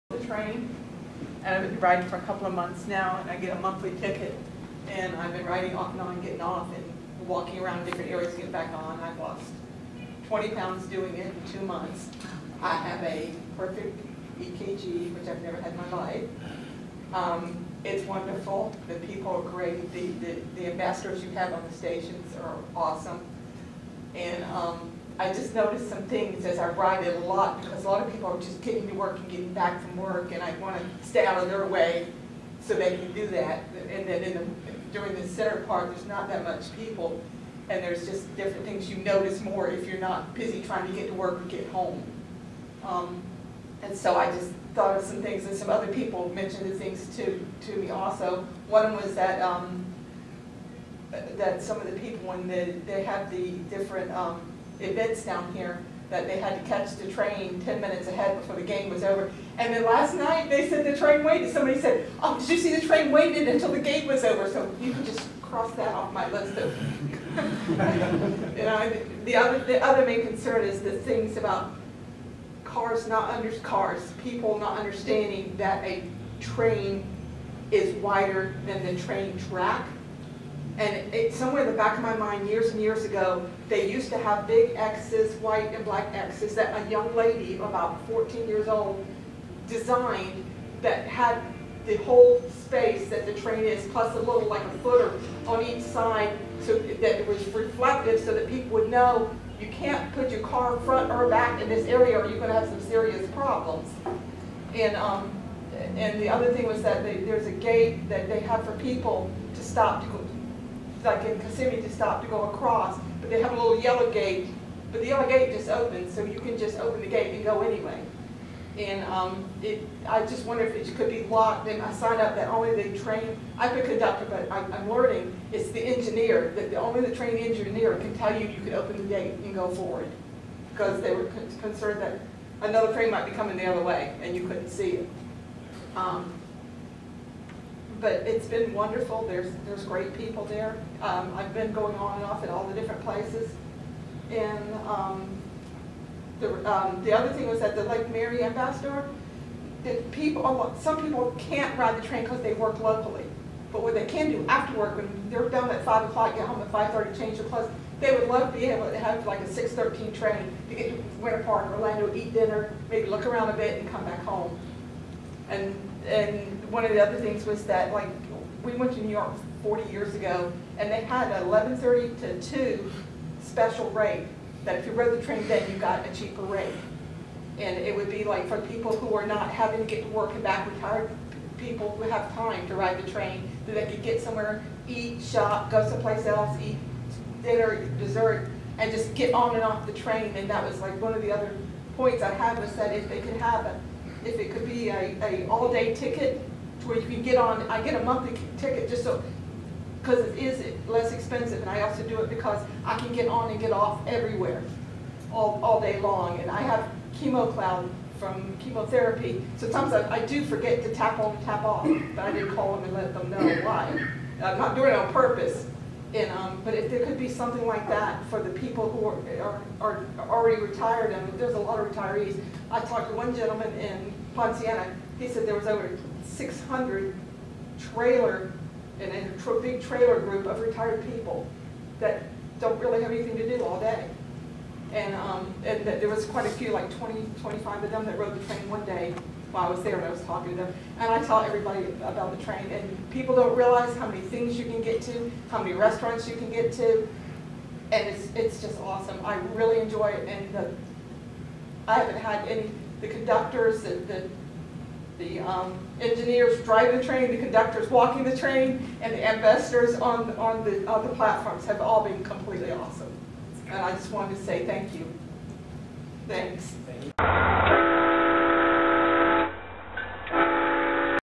Rider feedback on SunRail
A leisure rider tells SunRail brass what a good experience she had on the train.